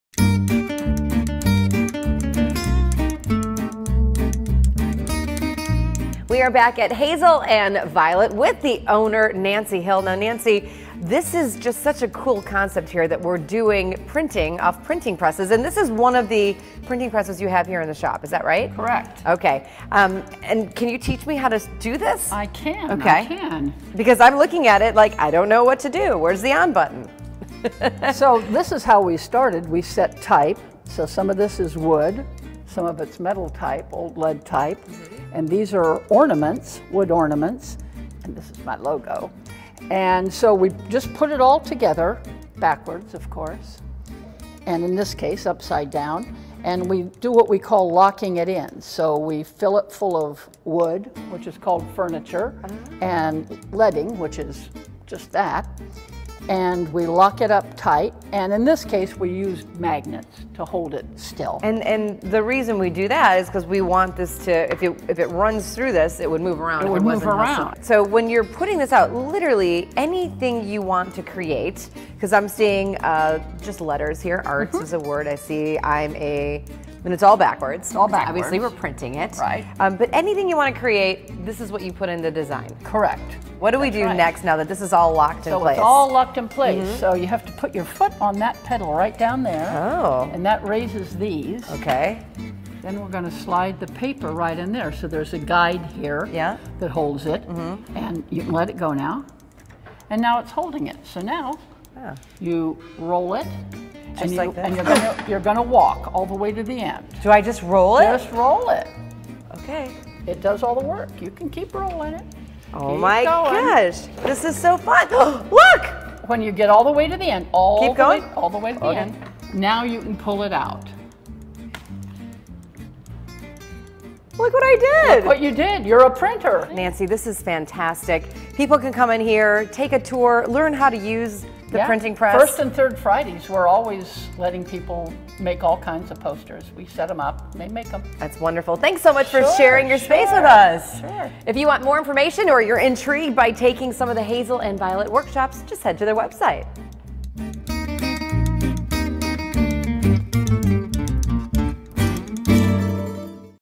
A Lesson in the Art of Letterpress Printing
Nancy Hill, the owner of Hazel and Violet gives Jane a lesson in the art of letterpress printing.